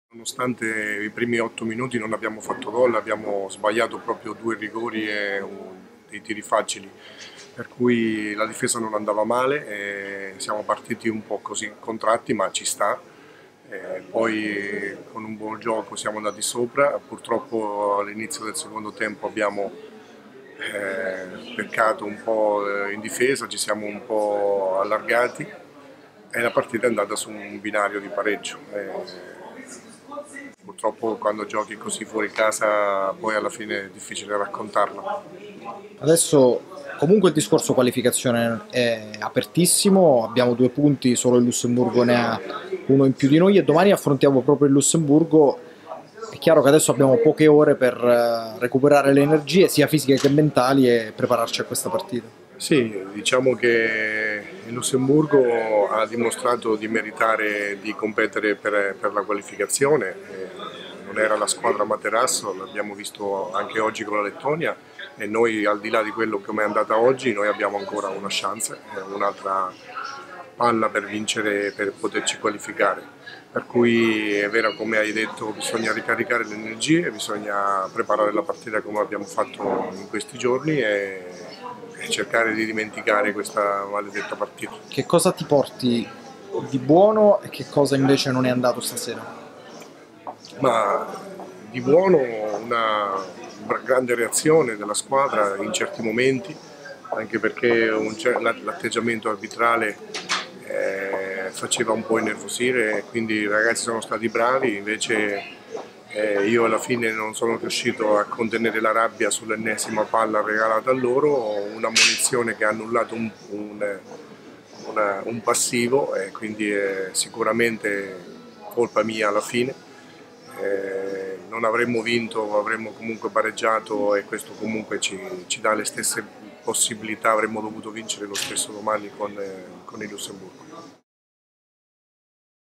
QUI ITALIA | Post-gara di Isole Faroe - Italia (27-26)
🔵 Azzurri superati a Tórshavn dalle 🇫🇴 Isole Faroe (27-26) nella seconda giornata del Gruppo 4 di qualificazione ai Mondiali 2023. Domani alle 16:30 sfida decisiva contro 🇱🇺 il Lussemburgo.

🎙 Il post-gara è affidato alle parole del DT Riccardo Trillini.